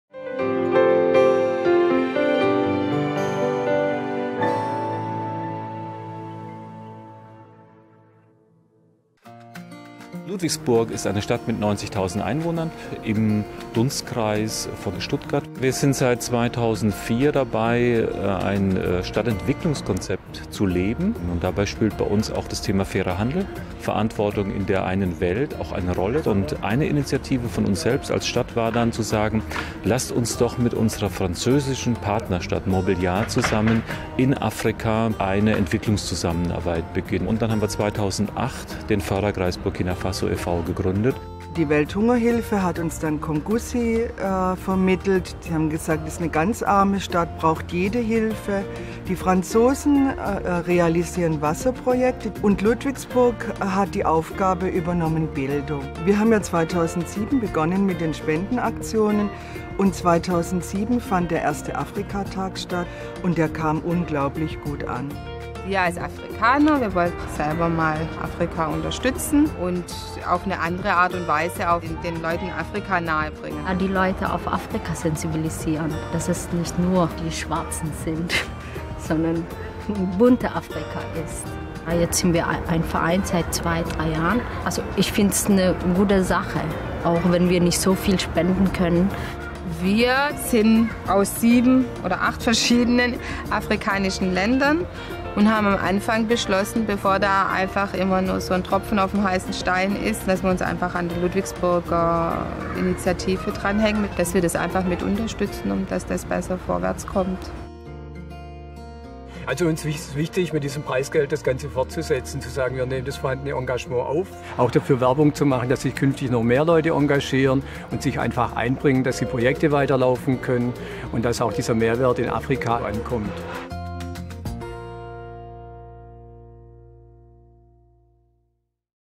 Wettbewerb: "Kommune bewegt Welt" - Preisträger Ludwigsburg
Am 15. September wurden die Gewinner des Wettbewerbs Kommune bewegt Welt geehrt.
Die Stadt Ludwigsburg erhielt 15.000€ Preisgeld.